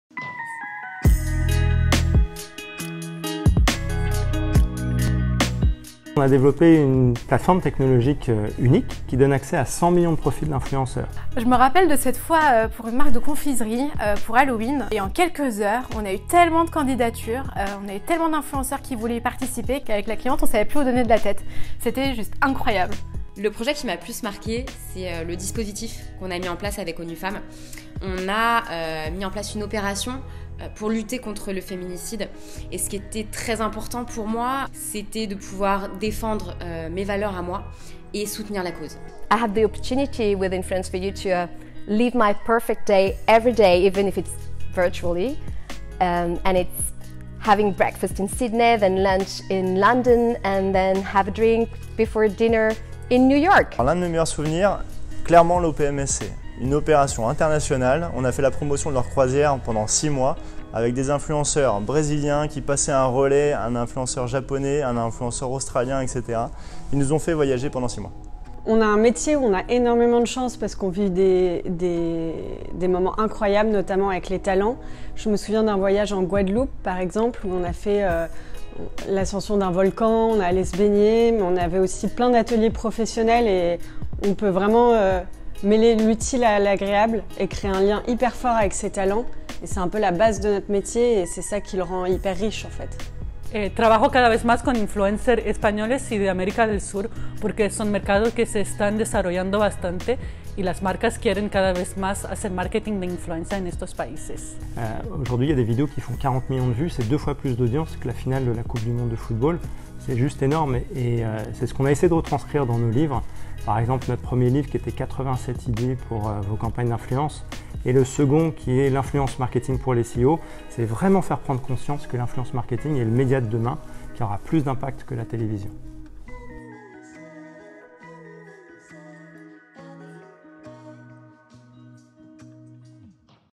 Influence4You - Hear from the team
Hear from our team! Florence, Stephane, Sebastien, Eva, Yassaman, Luana & Sophie tell us more about their job at Influence4You